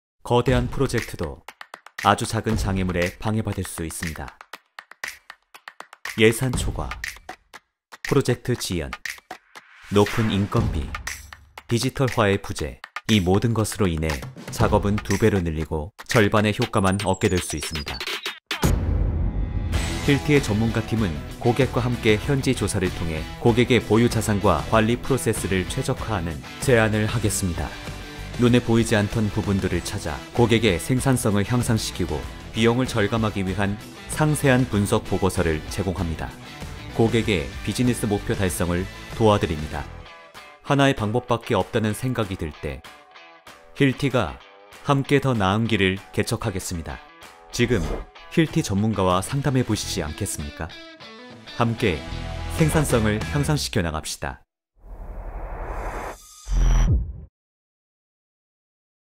【힐티코리아】 작업 현장의 생산성 향상, 힐티가 함께 합니다.
✌️ 힐티는 여러분이 현장에서 직면하는 문제에 대응할 수 있도록 도와 드립니다. 힐티는 신뢰할 수 있는 파트너로서 현장과 오피스에서 여러분의 생산성 개선을 위한 종합 솔루션을 제공합니다!

건설 프로세스의 시작부터 끝까지의 여정에 함께 하겠습니다.